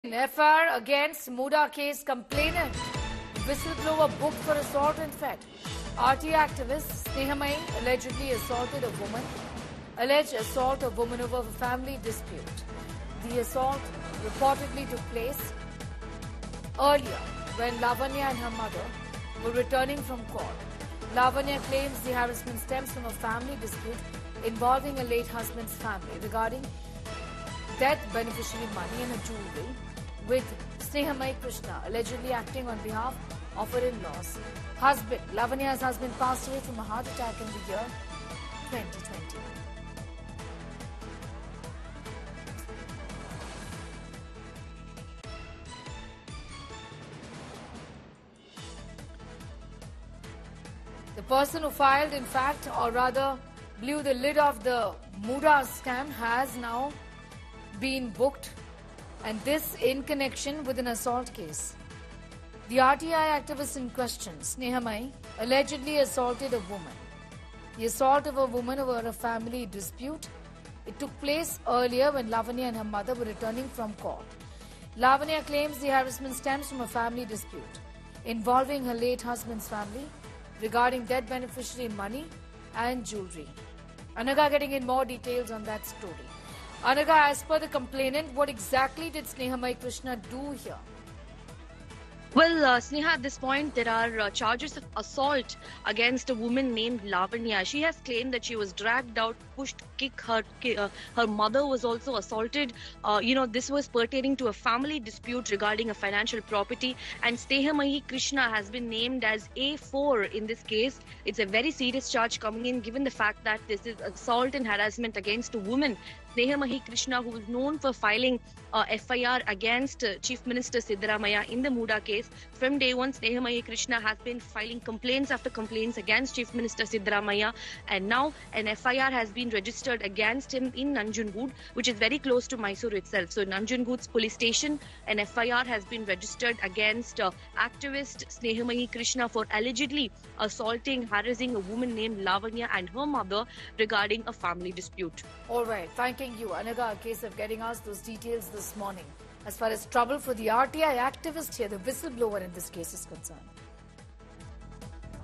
MUDA Complainant RTI Activist Snehamayi Krishna Booked for Assault and Theft | India Today
RTI activist Snehamayi Krishna, known for filing an FIR against Chief Minister Siddaramaiah in the MUDA case, has been booked for assault and theft. The charges stem from a family dispute involving a woman named Lavanya, who alleges that she and her mother were assaulted by Krishna. Lavanya claims the dispute is over debt beneficiary money and jewellery related to her late husband's family. An FIR has been registered against Krishna at Nanjungud Police Station, near Mysore.